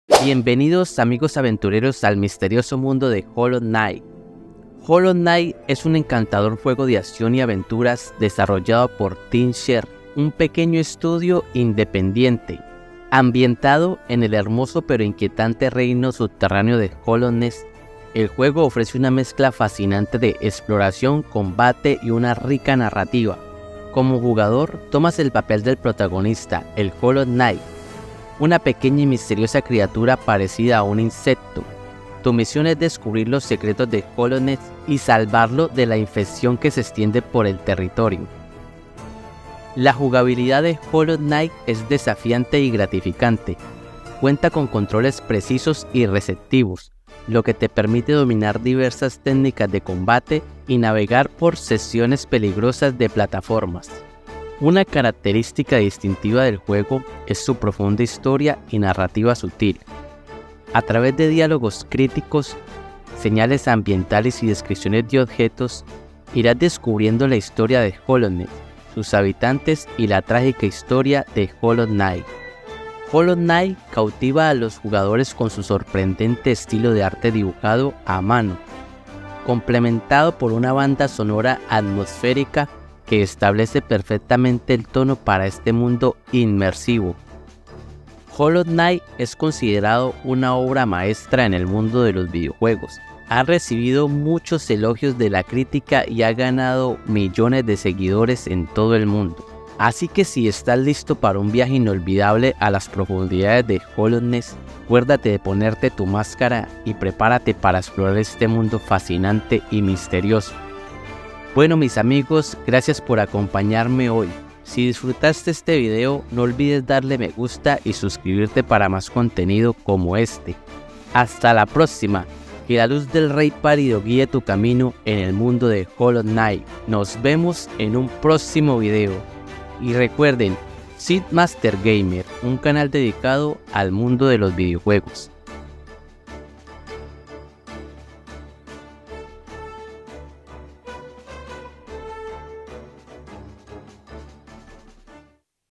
¿QUE ES HOLLOW KNIGHT?
"Adéntrate en el fascinante mundo de Hollow Knight, un cautivador juego de acción y aventuras desarrollado por el talentoso equipo de Team Cherry. Sumérgete en el hermoso, pero inquietante, reino subterráneo de Hallownest y prepárate para vivir una experiencia única llena de misterio y emociones.

En Hollow Knight, encarnarás al protagonista, un pequeño y misterioso insecto conocido como el Hollow Knight. Tu misión será descubrir los oscuros secretos que envuelven a Hallownest y detener la propagación de una insidiosa infección que amenaza con destruir este intrigante reino. A través de desafiantes combates y precisas plataformas, te enfrentarás a poderosos jefes y resolverás enigmáticos acertijos mientras te adentras en las antiguas ruinas de este mundo maravillosamente detallado.

Lo que hace que Hollow Knight sea verdaderamente especial es su cautivadora narrativa. A lo largo del juego, te sumergirás en la rica historia de Hallownest y sus habitantes, desentrañando sus oscuros pasados a través de diálogos crípticos, señales ambientales y descripciones de objetos. Cada descubrimiento será una pieza del rompecabezas que te llevará a entender la trágica historia del Hollow Knight.

Además de su profunda narrativa, Hollow Knight te cautivará con su impresionante estilo artístico dibujado a mano. Los escenarios cuidadosamente diseñados, llenos de detalles, cobran vida con una atmósfera única que te sumergirá completamente en este cautivador mundo subterráneo. Acompañando el arte está una evocadora banda sonora que resalta la experiencia, brindando emociones y sensaciones mientras exploras este intrigante reino.

Hollow Knight ha recibido elogios de la crítica y ha enamorado a una dedicada base de seguidores en todo el mundo. Si estás buscando una aventura desafiante y cautivadora, no puedes perderte la oportunidad de adentrarte en los misterios y maravillas de Hollow Knight. Ponte tu máscara y prepárate para emprender un viaje inolvidable en busca de la verdad en este fascinante y misterioso mundo."